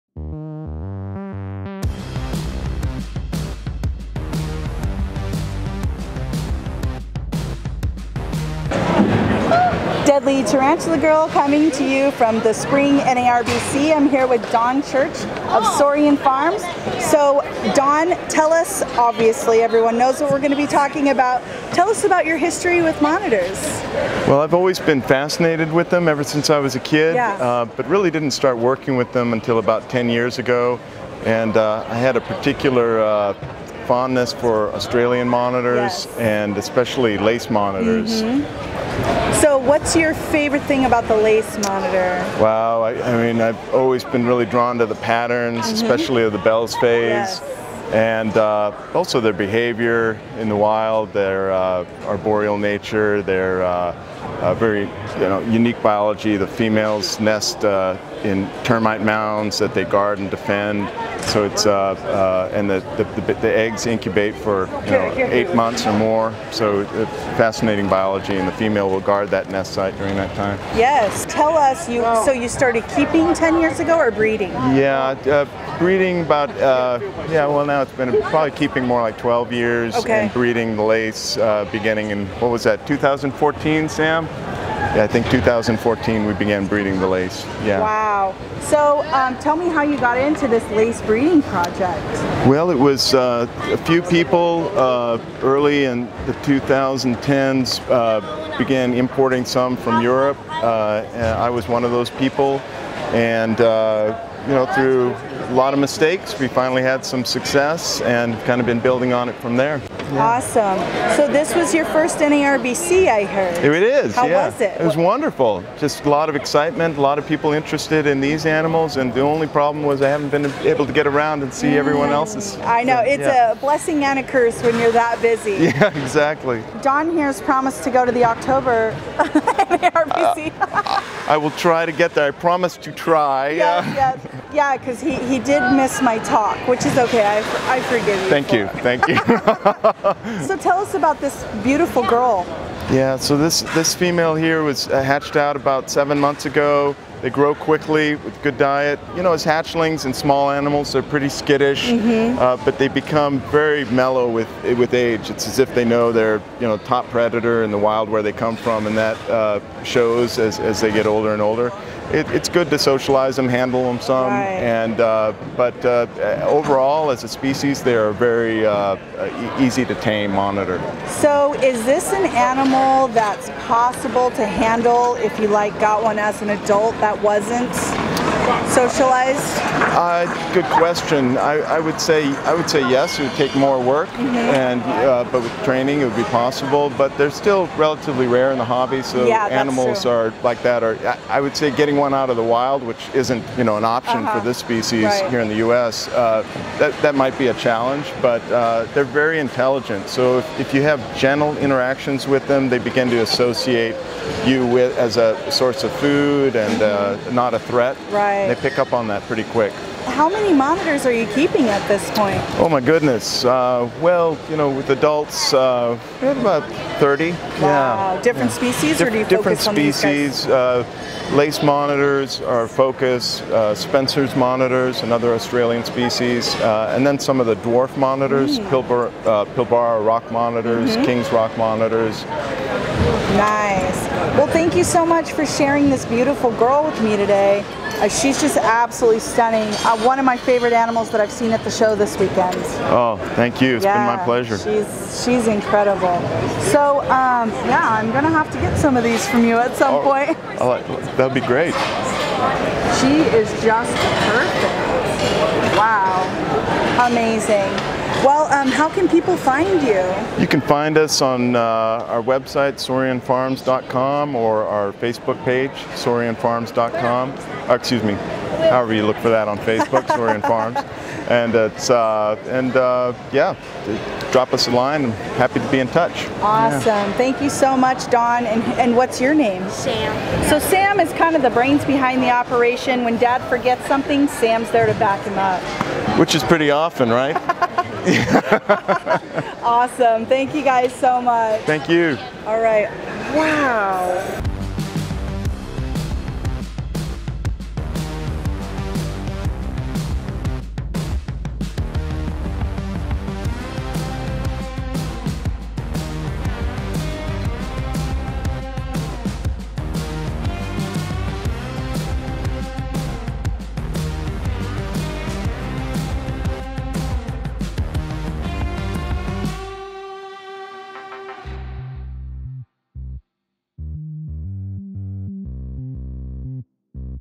Australian Belli Phase Monitor: Rare and Beautiful
It's not actually pied, but the Belli Phase of the Lace Monitor, Varanus varius.  This is one of my favorite species that I used to only see in books and now, I actually got to hold one.  I'm in love:)  Be sure to check out Saurian Farms: